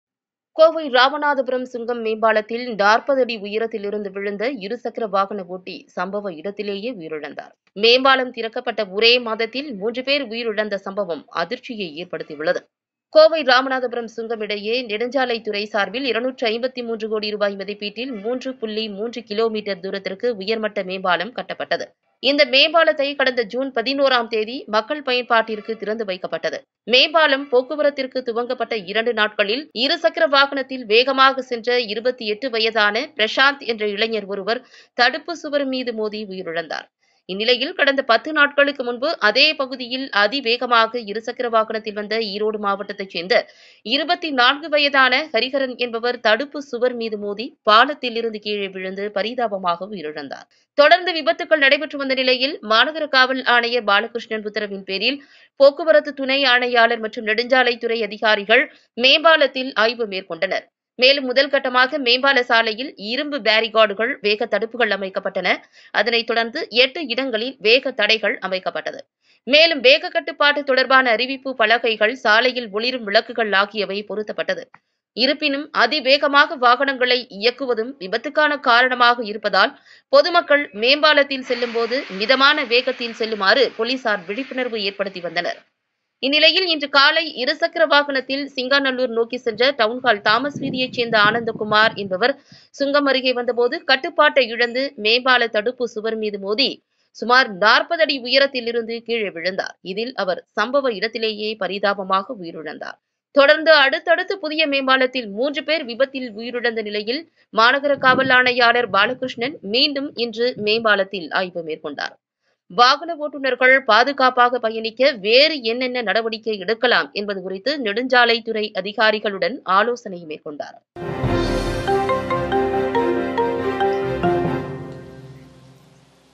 மீண்டும் வாகன விபத்தில் ஒருவர் பலி..! கோவை ராமநாதபுரம் மேம்பாலத்தில் தொடரும் விபத்துகள்
Third accident in a row: Man killed in bike accident on Ramanathapuram new flyover in Coimbatore